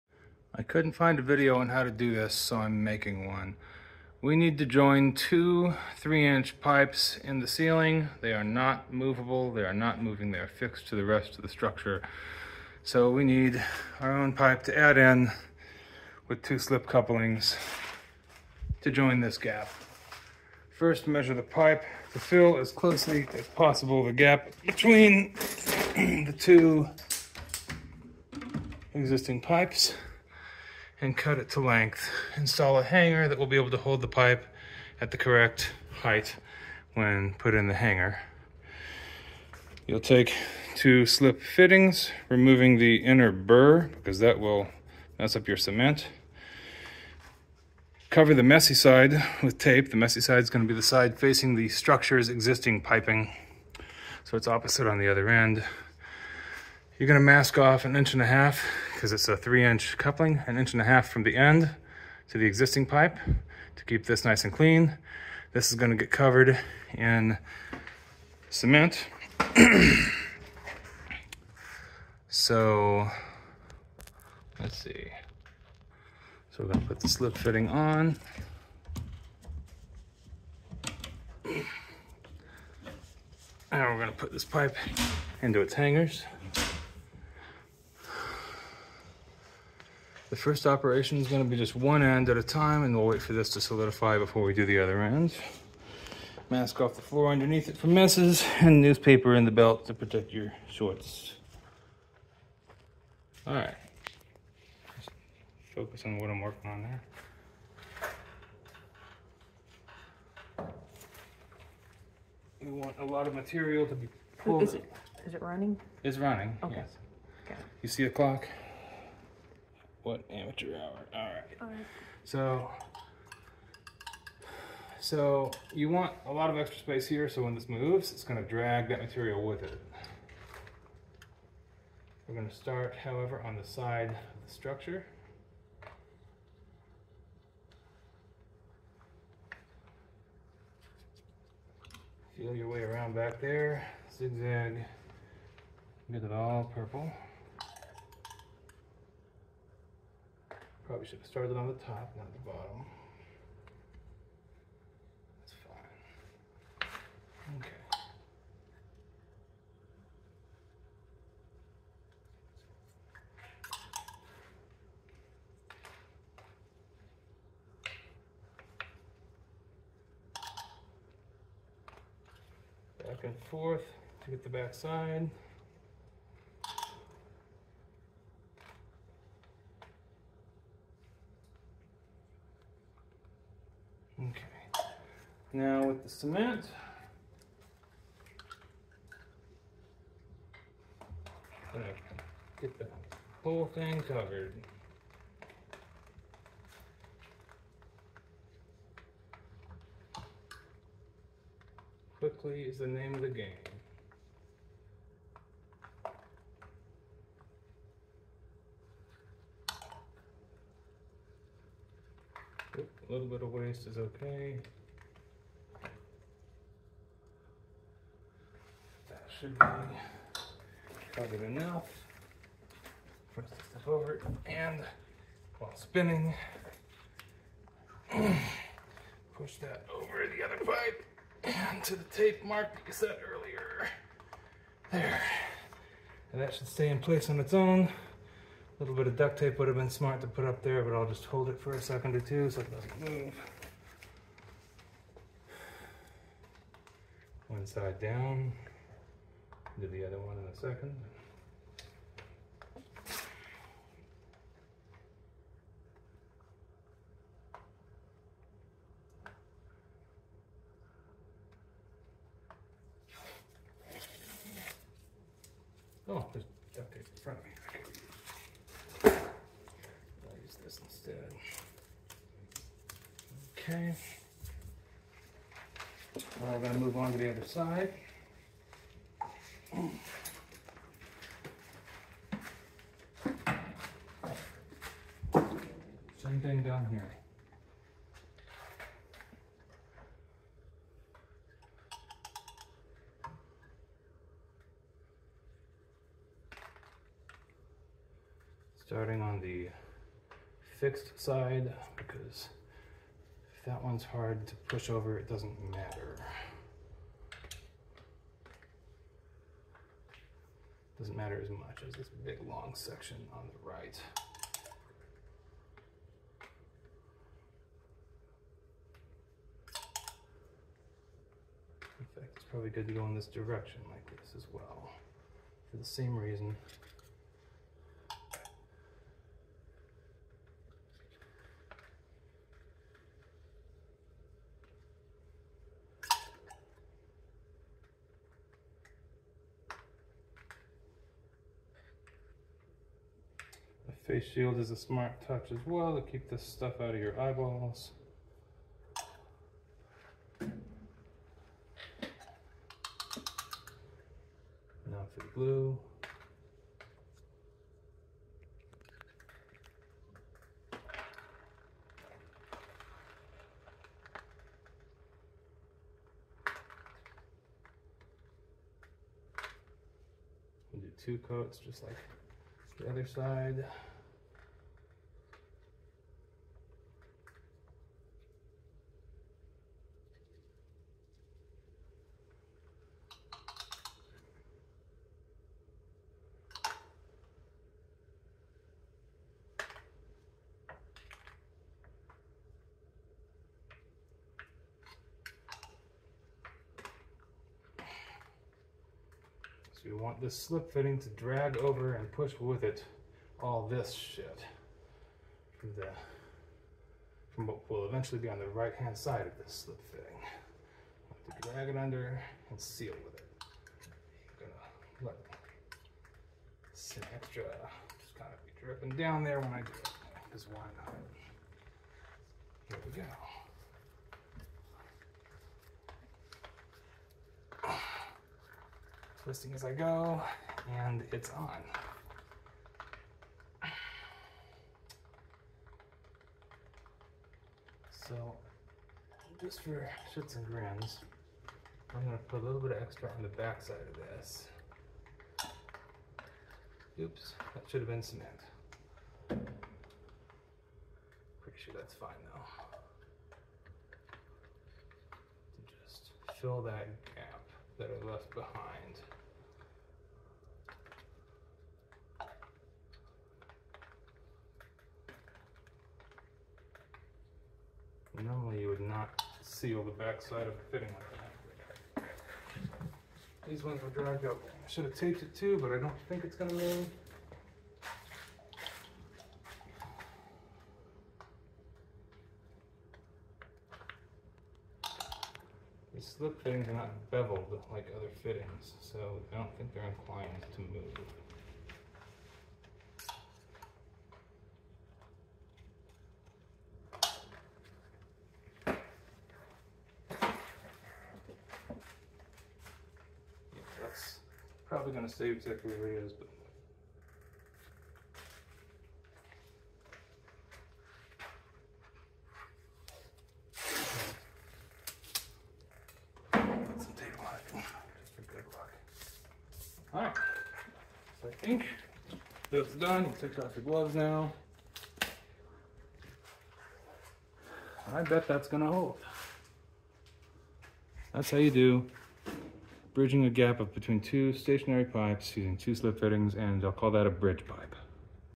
[FAIL] Joining two stationary 3" PVC DWV pipes with double slip couplings (mistakes in description)
Mistakes:
1) I missed some spots with primer. SHOULD HAVE A MIRROR ON HAND or otherwise inspected 360 before proceeding to cement.

2) I forgot to give it a spin after the coupling is in place. would give it a 1/3 turn after it's in place to help spread cement.

3) I misidentified the cans and added primer on the left coupling edge after placement when I meant to add only cement

4) I should have dry fit the left side while doing the right side to guarantee alignment, and actually waited for right side to cure before doing left side.

5) I should have removed masking tape right after I was done with the cement to get a nice clean line

Video omissions:
You should take time to dry fit first, adjusting alignment to perfection and adding shims to the supports where necessary. use a long straight-edge on various sides of the pipe to make it as straight as possible.  A vertical slope is expected and important, but this line was not constructed horizontally straight with the joist either. so every hanger is a different offset from the joist.

The higher of to two joints began dripping 48 hrs after this video.  We ended up using NSF no-hub rubber couplings with steel pipe clamps. If you know a better way of doing it, make a video of your work and link to it in a comment here.